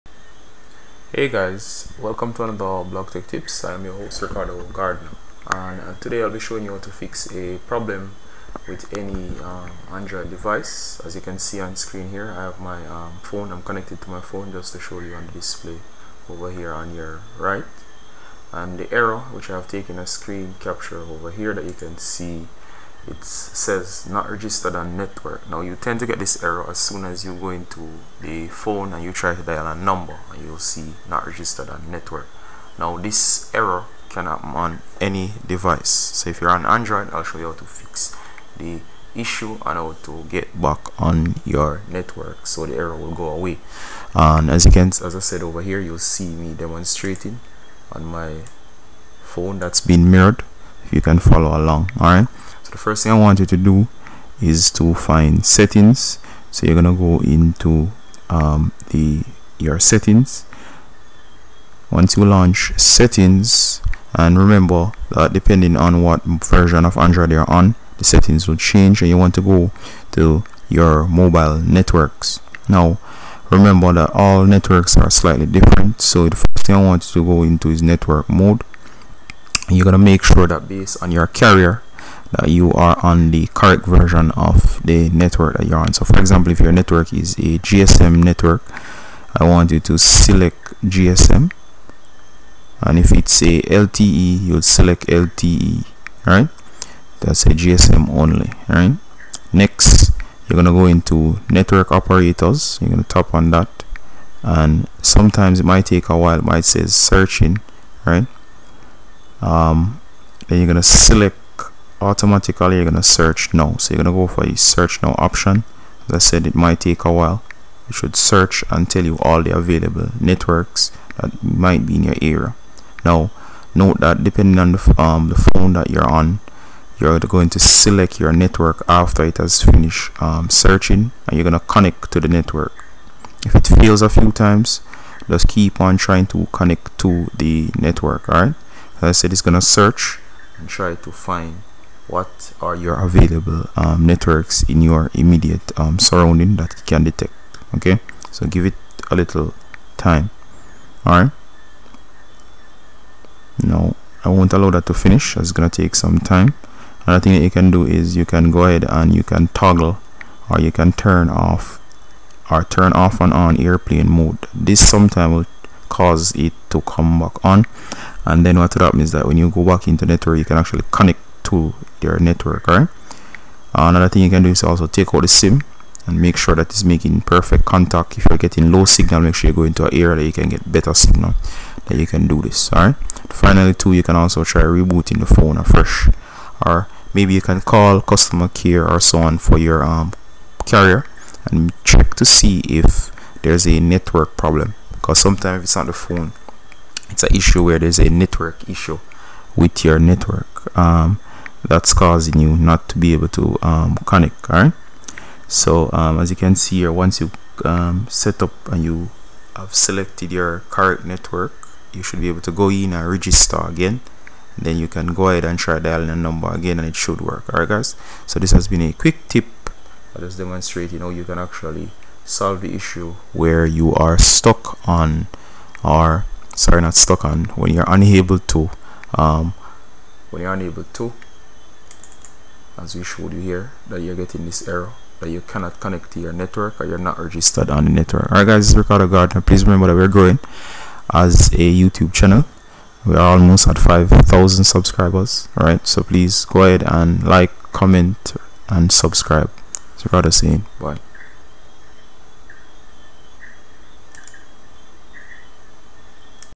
Not registered on network
If you get the error Not registered on network then you are not alone, try the following to fix the issue and return to making calls again on any network.